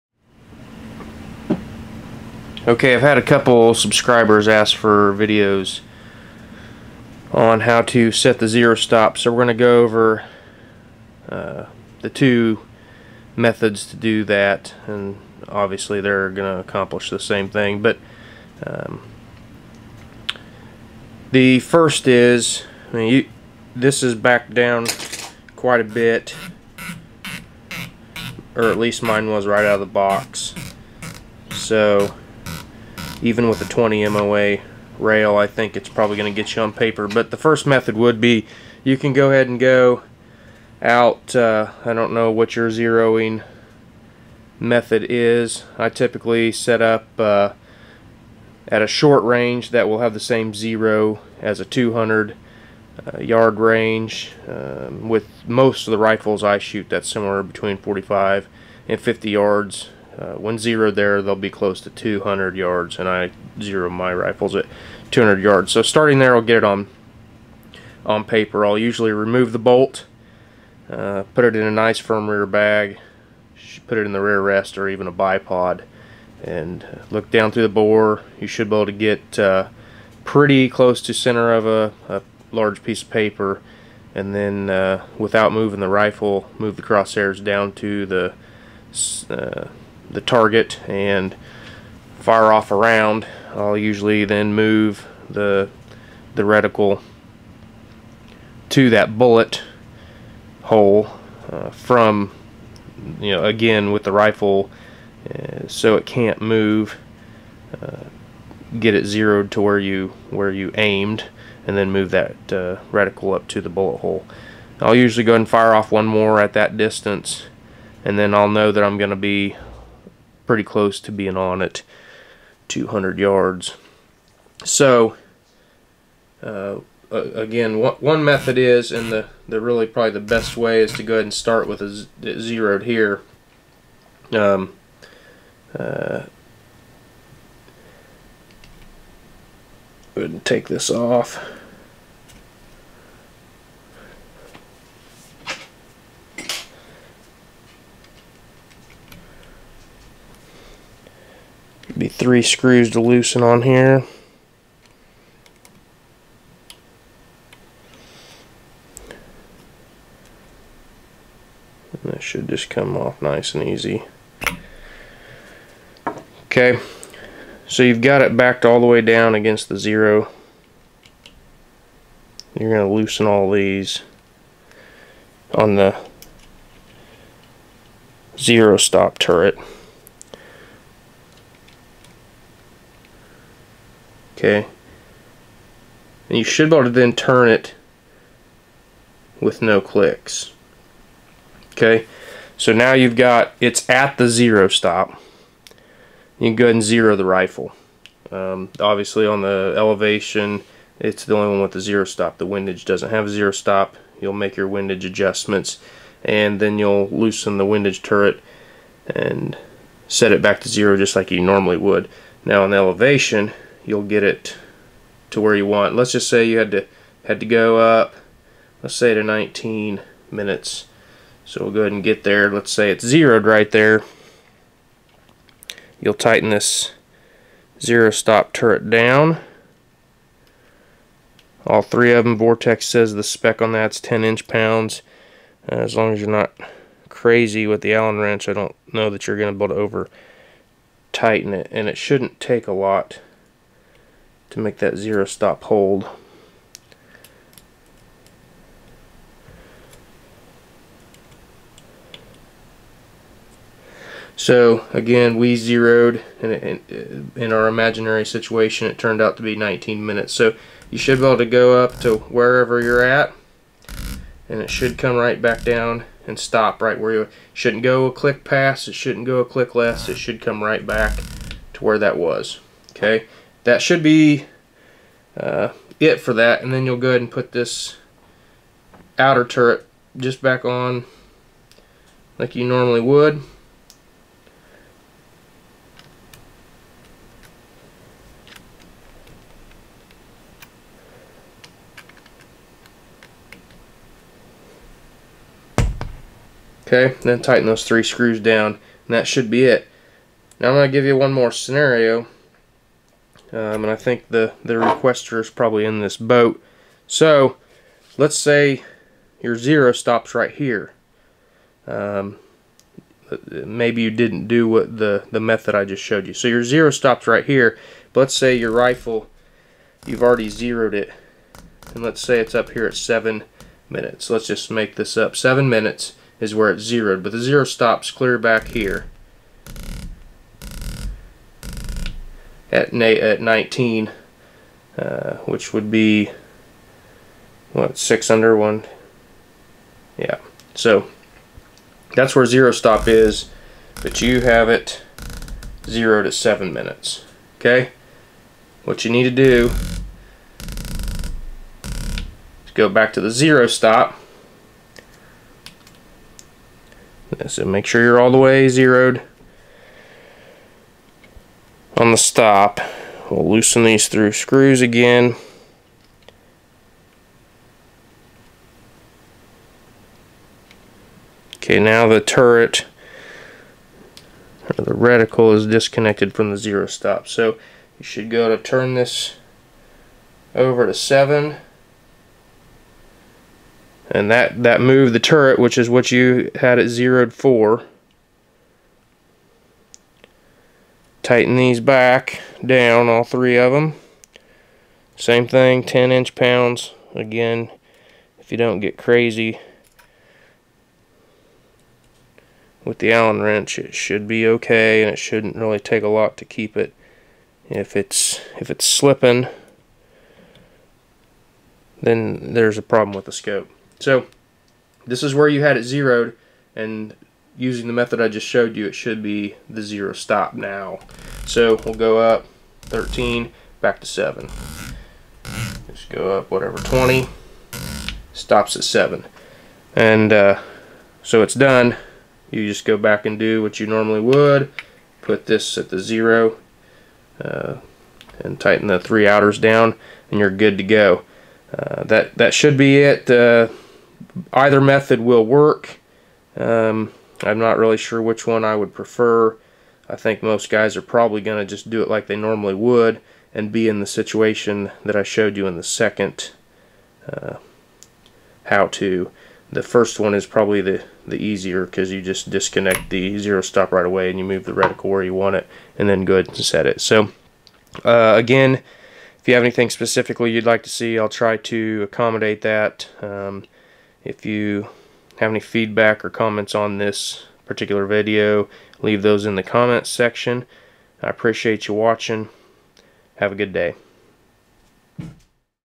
Setting Vortex Gen II PST zero stop
How to set the zero stop on your Gen II Vortex PST.